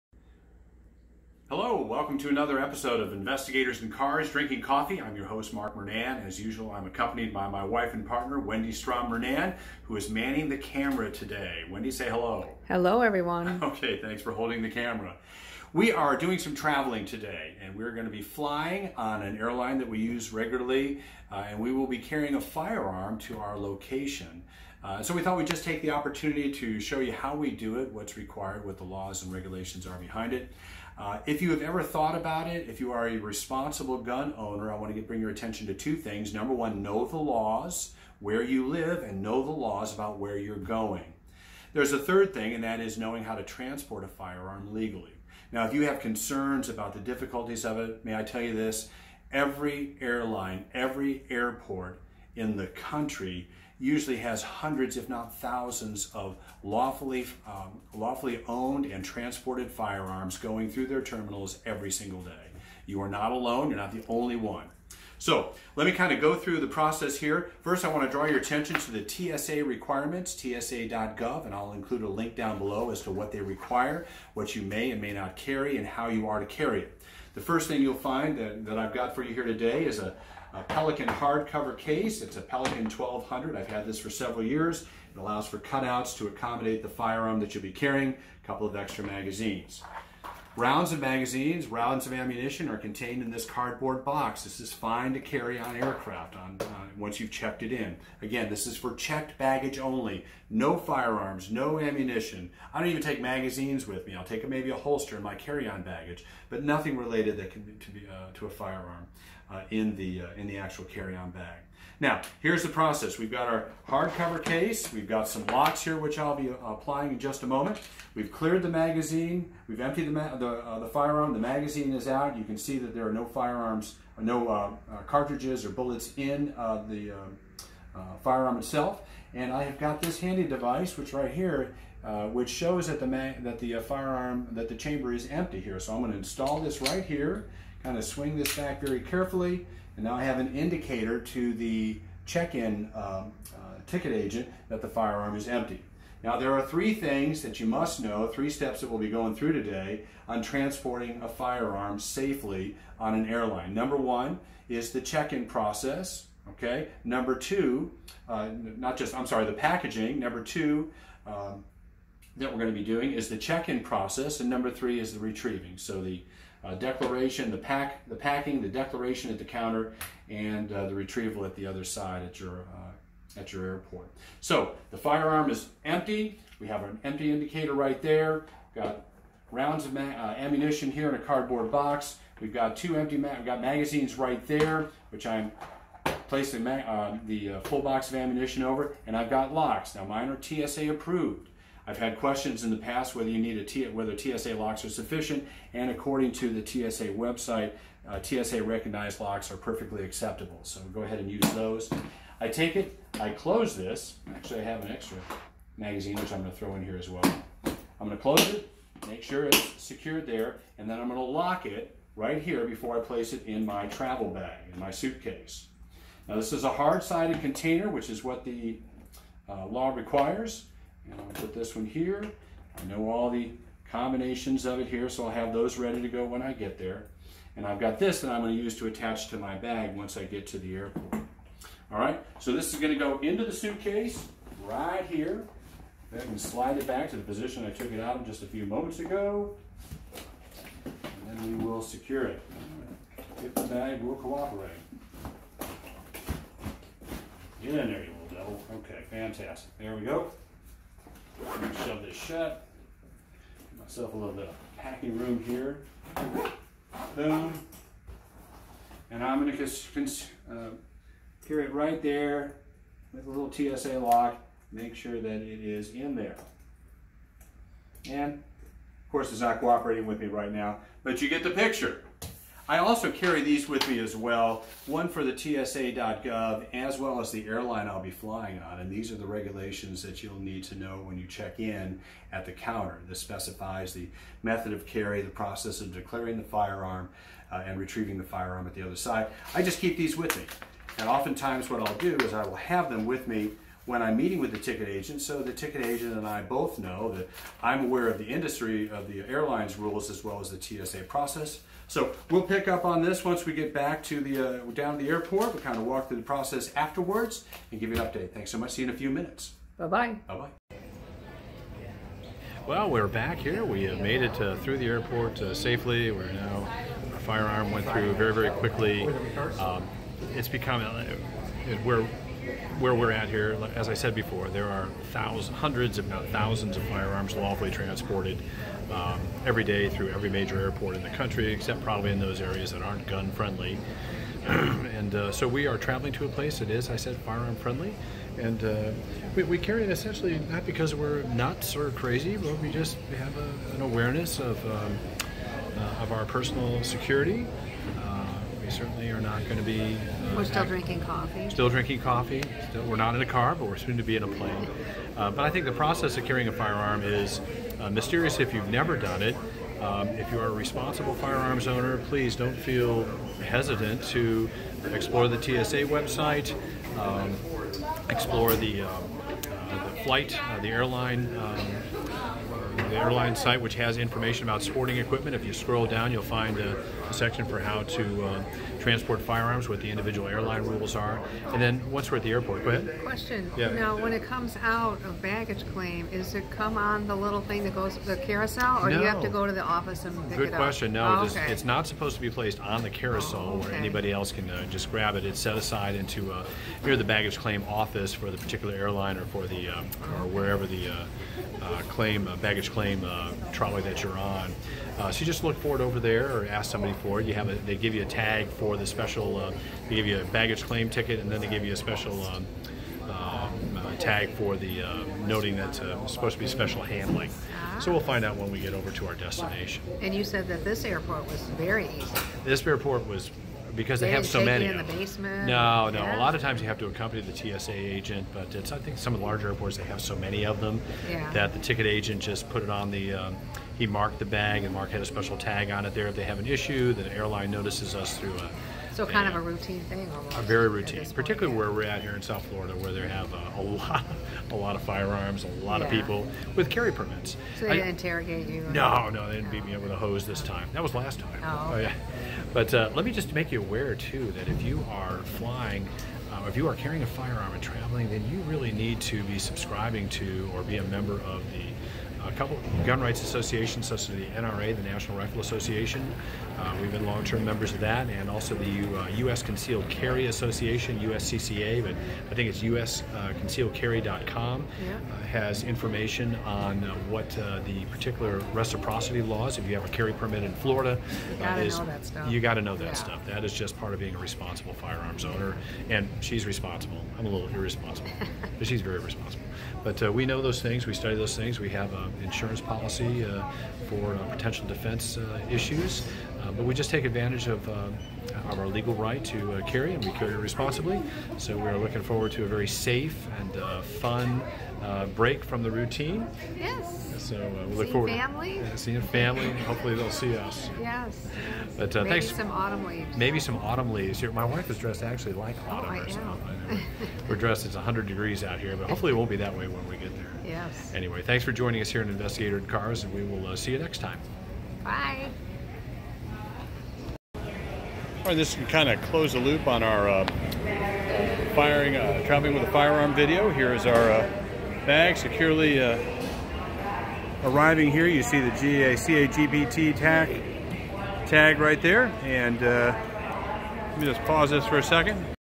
Flying with a Firearm
Are you a "responsible" firearm owner? Then you need to know how to legally transport your firearm in compliance with state and federal law! Our investigators get out of the car and head to the airport for some holiday travel, and pack their "EDC" pursuant to airline and TSA regulations. Join us as we show you the proper way to pack, declare and reclaim your firearm!